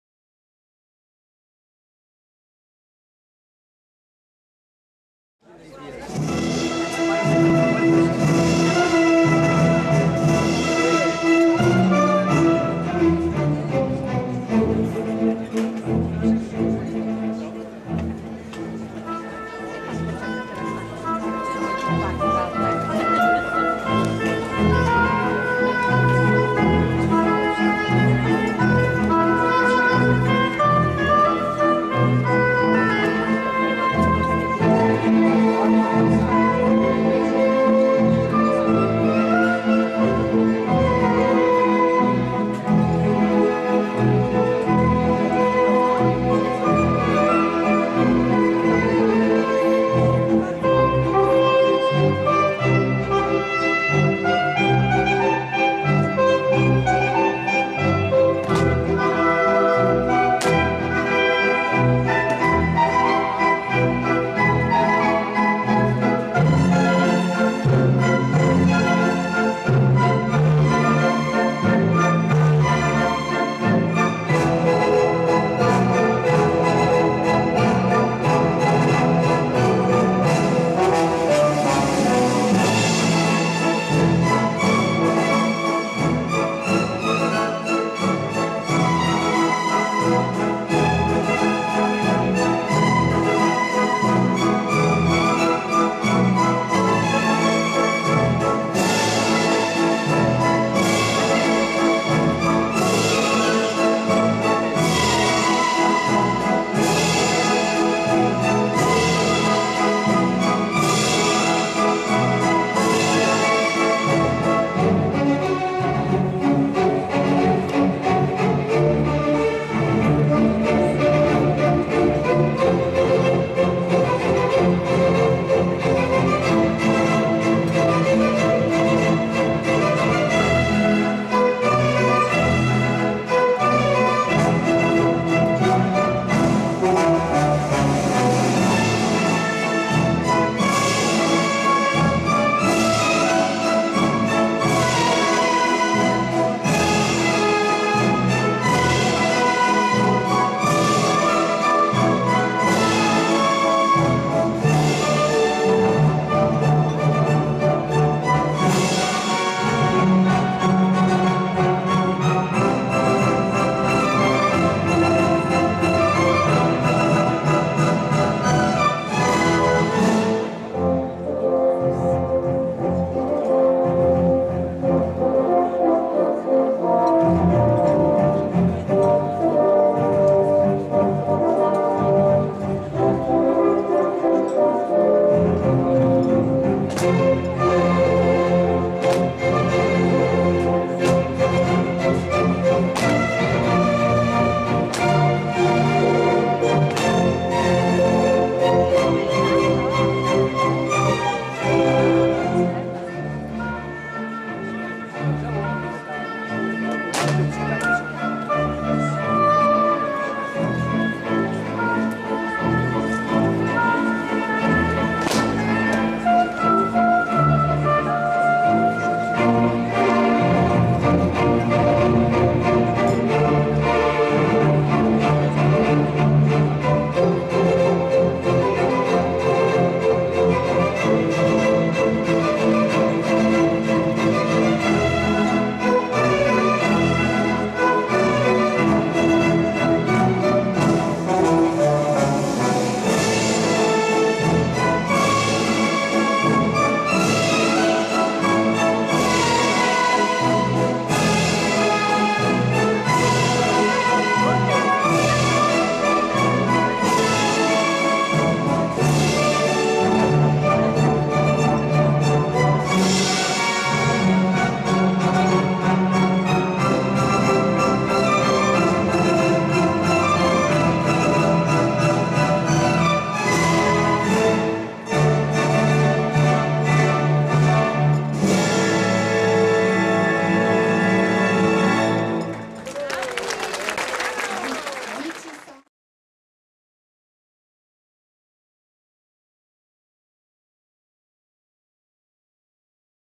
Wici Dancers -- 2010 PAC Illinois Division -- Part 2
The Wici Dancers performed the elegant Polonaise at the Heritage Awards Dinner of the Illinois Division of the Polish American Congress (PAC) on October 29, 2010. The event took place at White Eagle Banquets in Niles, Illinois, a suburb of Chicago.